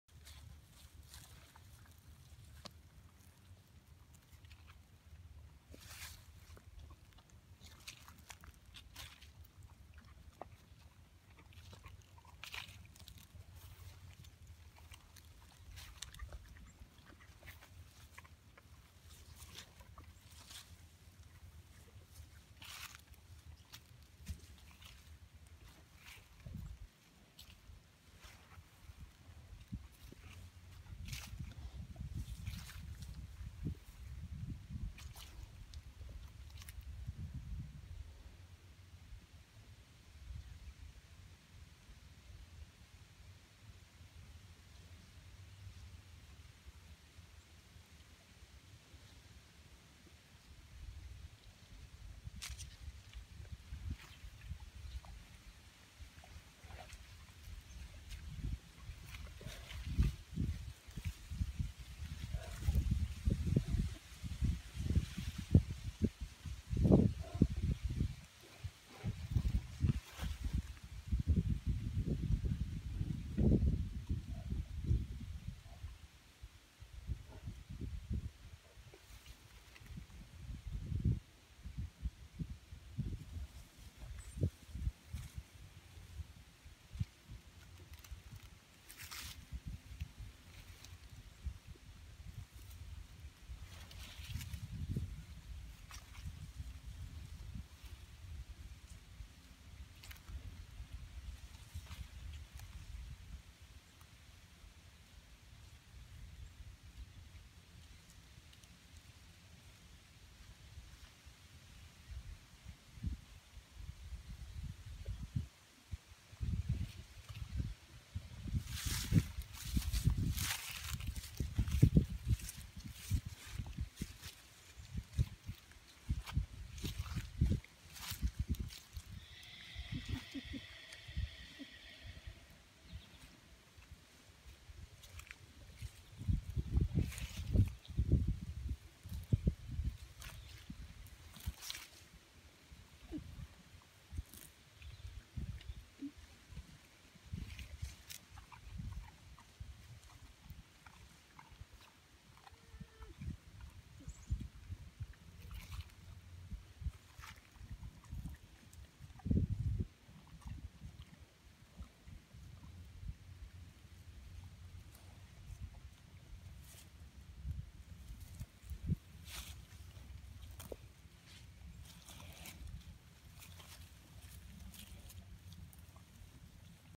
Bears grazing
Yesterday may have been the last nice sunny and warm day we have this year, as today the water bowl is frozen. The bears took advantage of it. Note the two squirrel nests in the trees (one on top of the other). Our released squirrels I’m sure!